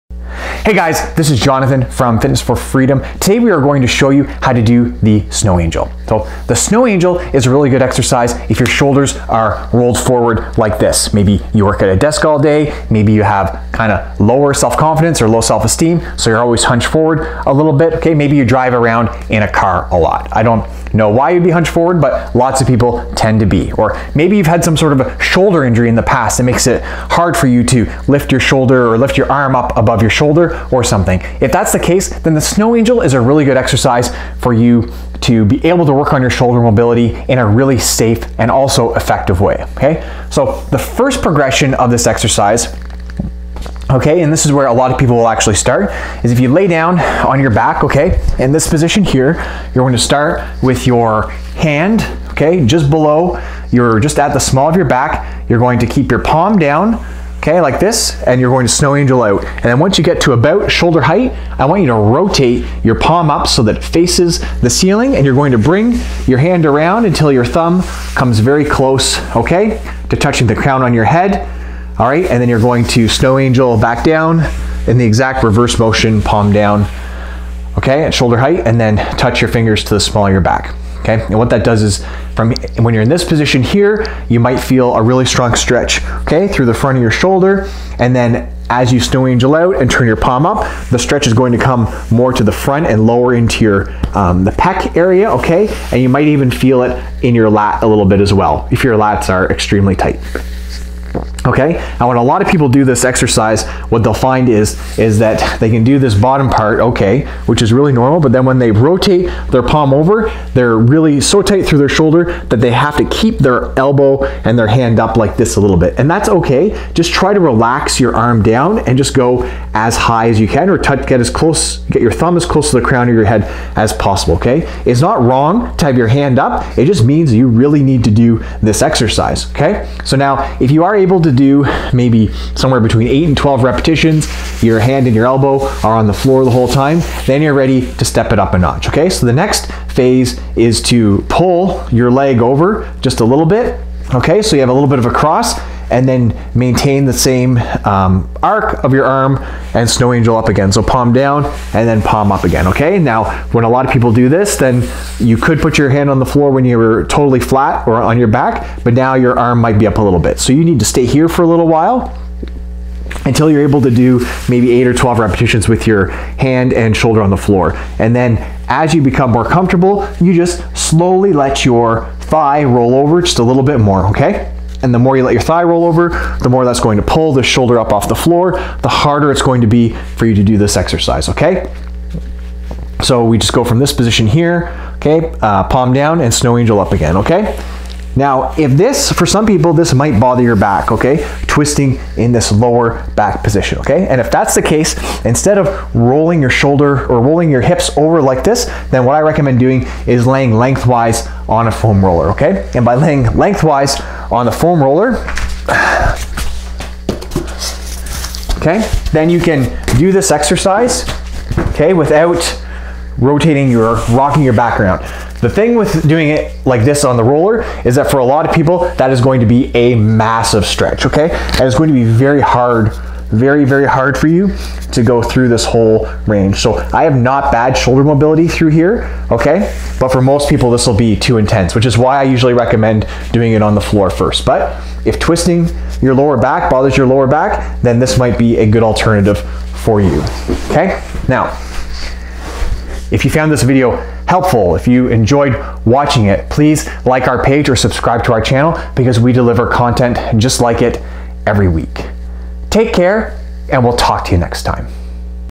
Improve Shoulder Mobility - Snow Angel Exercise
Shoulder mobility is really important, it let's you strengthen your shoulder through it's full range of motion, it helps make shoulder exercises more effective, it can decrease shoulder pain and it can help you improve your posture. The snow angel exercise is one of my favourites to improve your shoulder mobility, because there's a progression for everyone.

The Perfect Posture Program
iOS